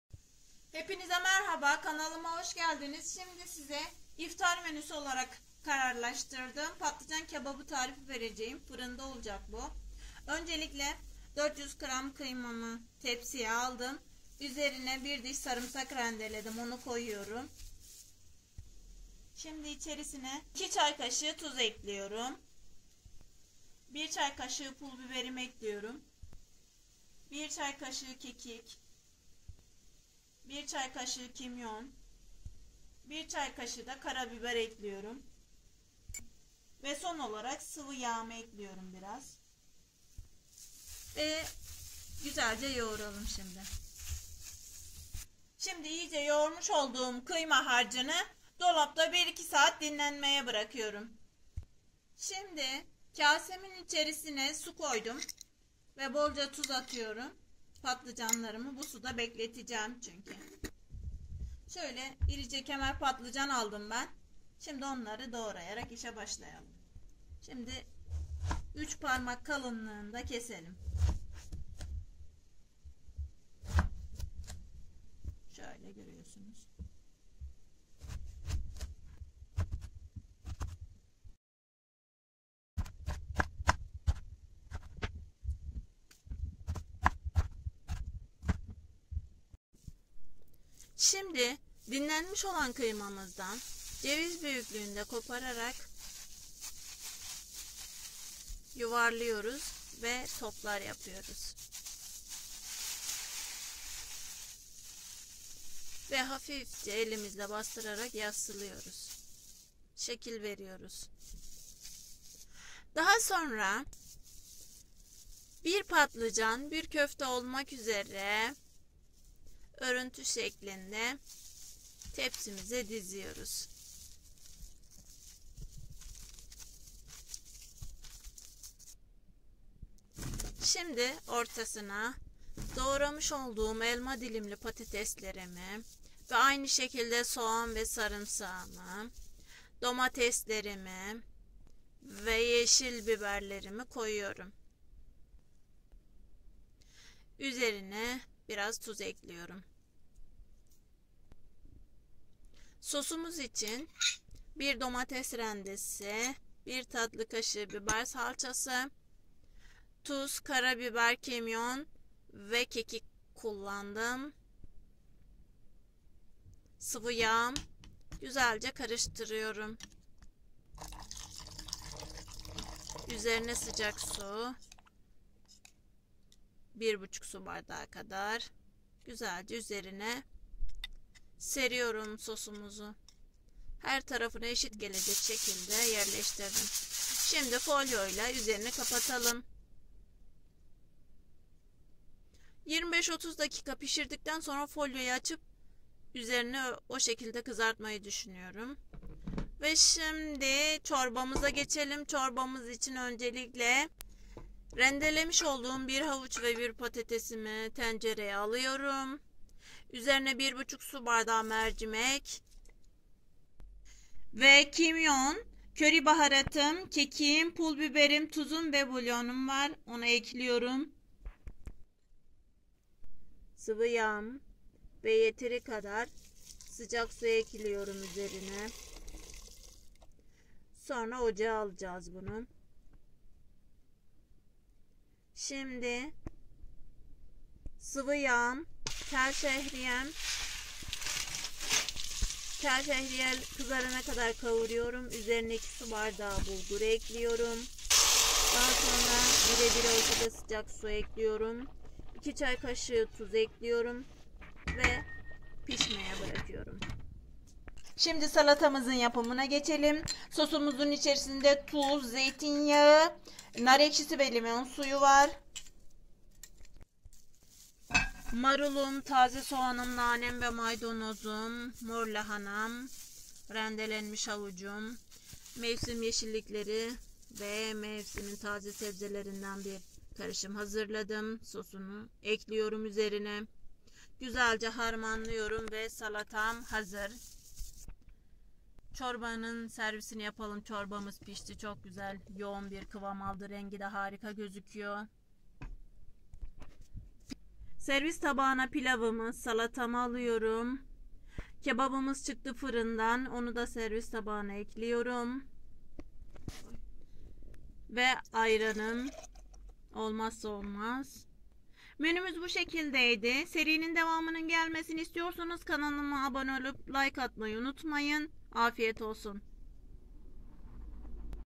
PATLICAN KEBABI + MERCİMEK ÇORBASI + PİLAV SALATA İftarmenüsü 1
İftar Menüsü :
Patlıcan kebabı 00:09
Mercimek çorbası 03:18
Bulgur pilavı 03:55
Mevsim salatası 04:17